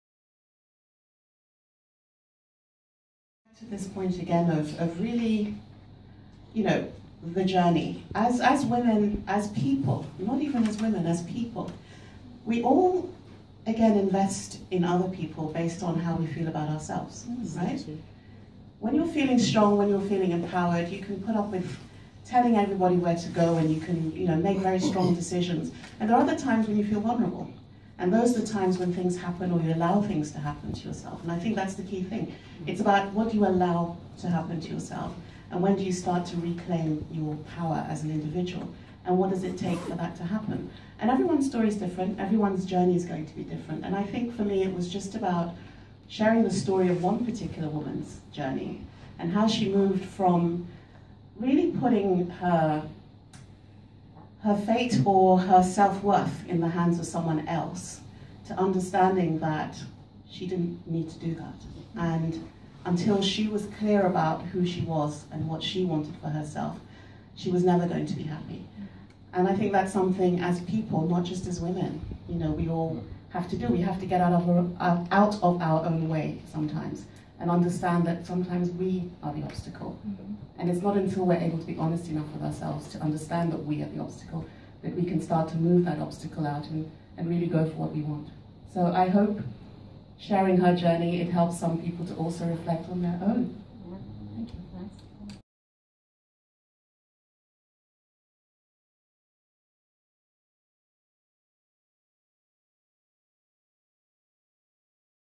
Frances Mensah Williams on Love, Power & Happiness
Frances Mensah Williams, author of summer bestseller From Pasta to Pigfoot, talks about women, empowerment and the journey to happiness at the Brand South Africa WeAreAfrica event celebrating AfricaDay 2015